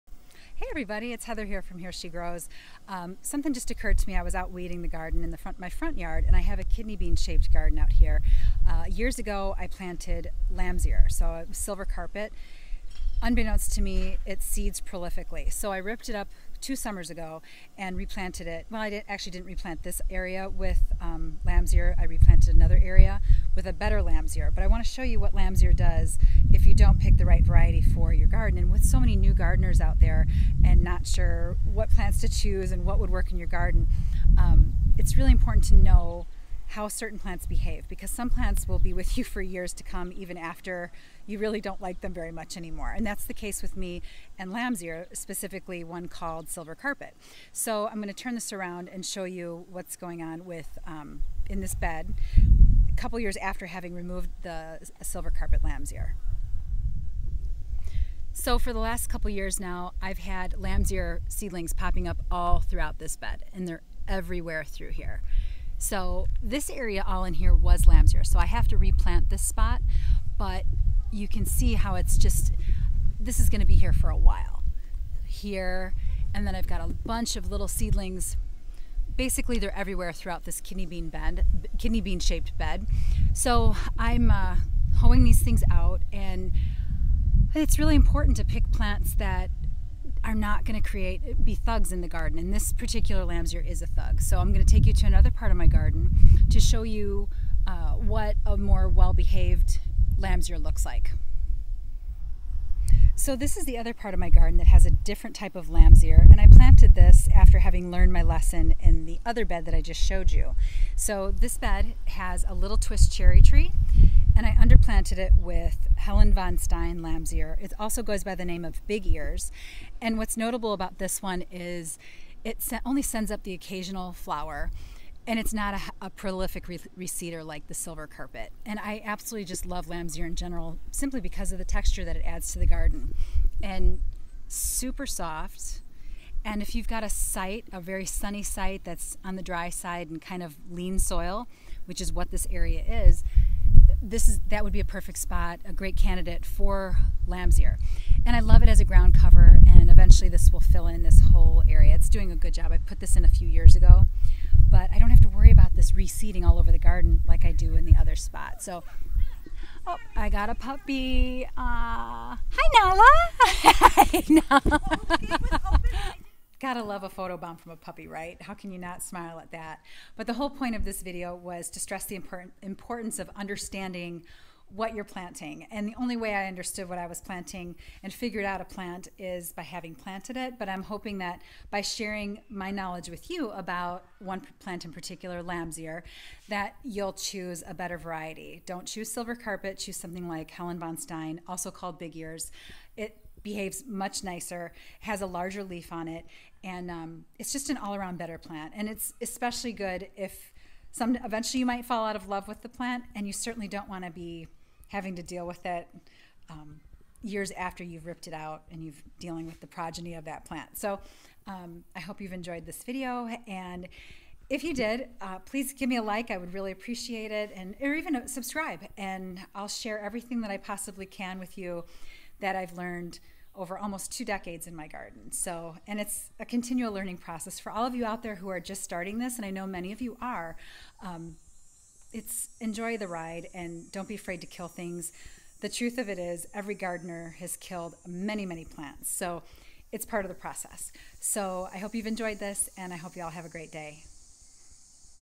Choose the Right Lamb's Ear | Here She Grows
Not all lambs ear are equal. Some play nicely in the garden while others take over, reseeding everywhere in an attempt to be the only plant in the garden. Here’s what you need to know when choosing one of these drought tolerant, silver-leaved beauties.